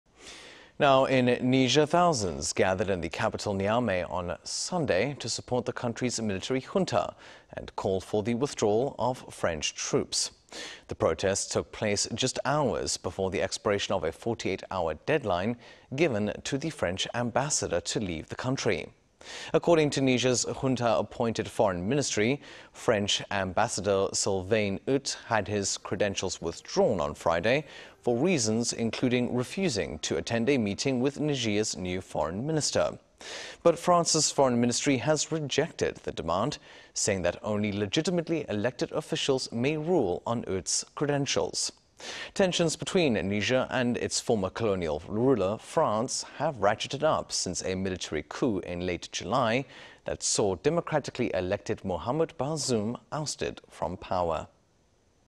Protests in Niger with French ambassador's 48 hour departure deadline set to expire
니제르 군부 "프랑스 대사 48시간 내로 떠나라"

In Niger, thousands gathered in the capital Niamey on Sunday to support the country's military junta and call for the withdrawal of French troops.
The protests took place just hours before the expiration of a 48-hour deadline given to the French ambassador to leave the country.
According to Niger's junta-appointed Foreign Ministry, French ambassador, Sylvain Itte had his credentials withdrawn on Friday, for reasons including refusing to attend a meeting with Niger's new foreign minister.
But France's foreign ministry has rejected the demand saying that only legitimately elected officials may rule on Itte's credentials.
Tensions between Niger and its former colonial ruler France have ratcheted up since a military coup in late July that saw democratically elected Muhamed Bazoum ousted from power.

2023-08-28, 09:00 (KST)